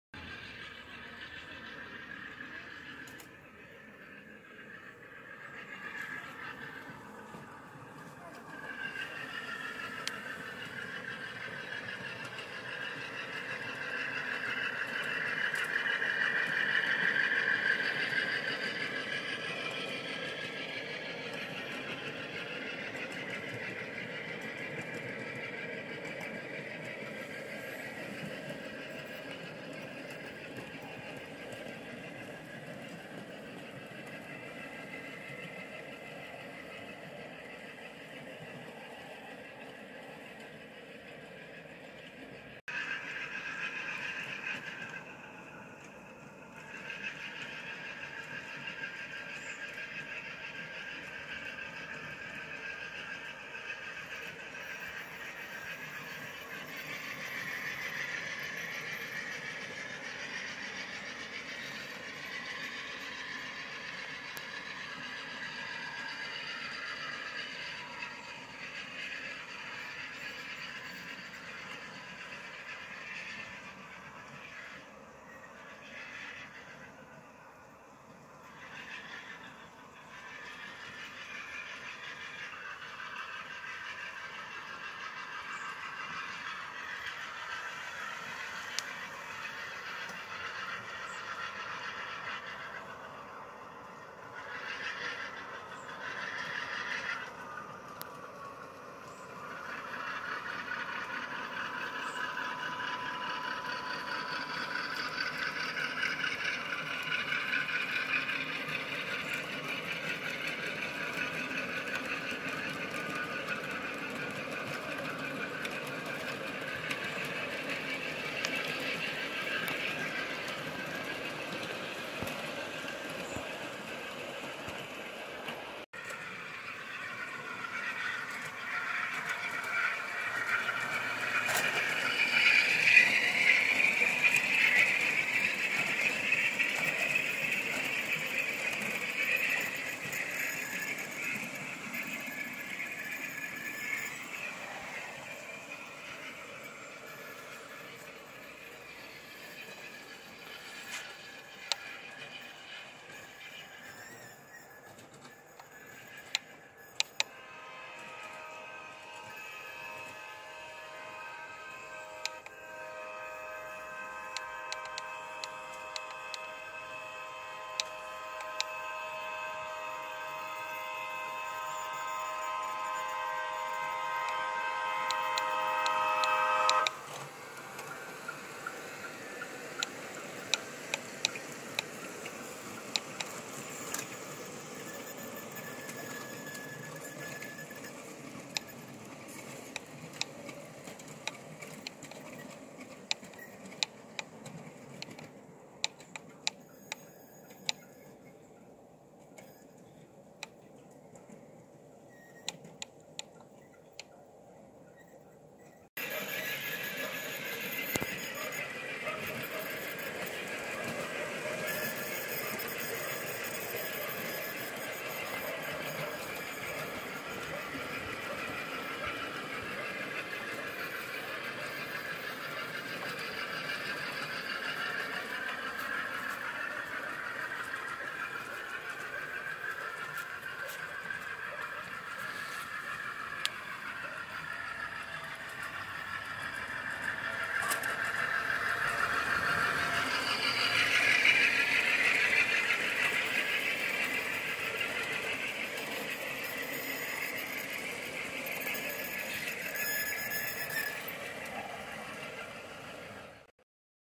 Aristo Dash-9 with TCS sound on USA Trains BN and Alberta 4-bay grain hoppers
Just having some fun running some trains in the garden prior to Christmas